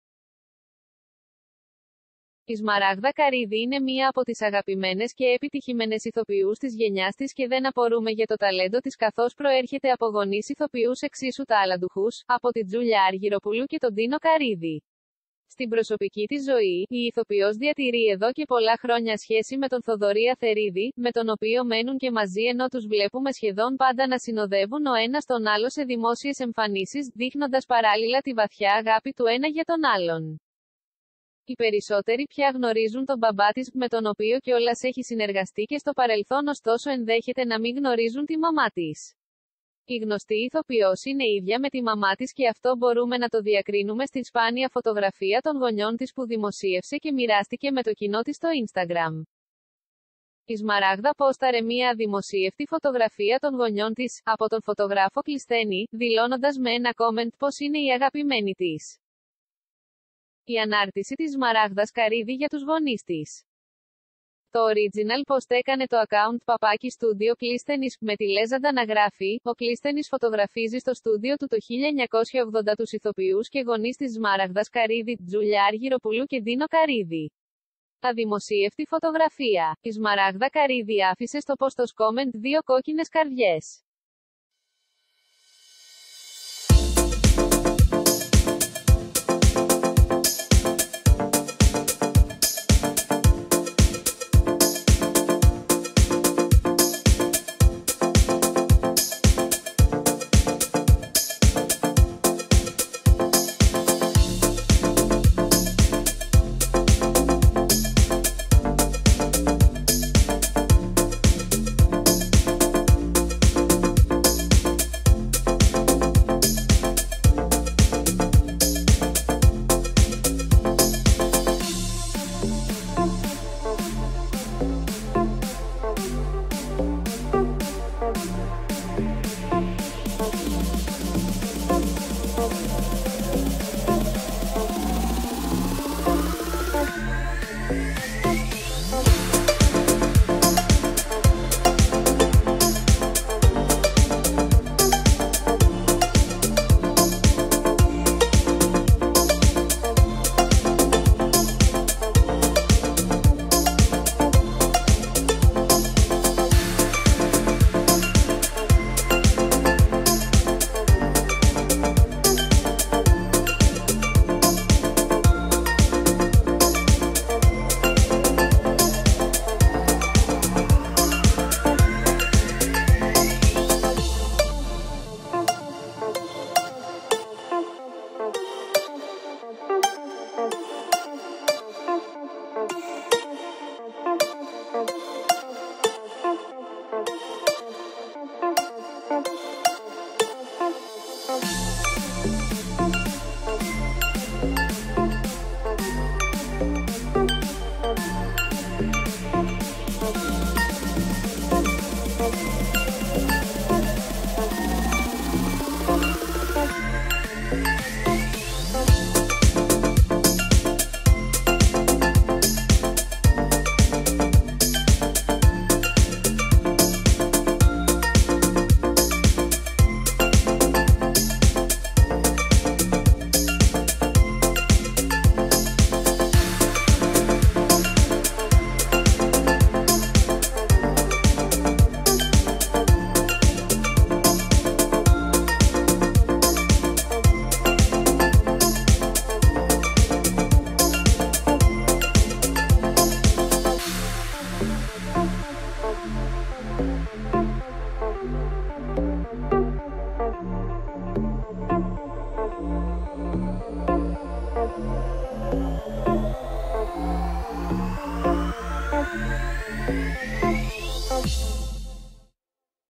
Σμαράγδα Καρύδη: Σπάνια φωτογραφία της μητέρας της - Απίστευτη ομοιότητα
Η Σμαράγδα Καρύδη είναι μία από τις αγαπημένες και επιτυχημένες ηθοποιούς της γενιάς της και δεν απορούμε για το ταλέντο της καθώς προέρχεται από γονείς ηθοποιούς εξίσου ταλαντούχους, από τη Τζούλια Αργυροπούλου και τον Ντίνο Καρύδη.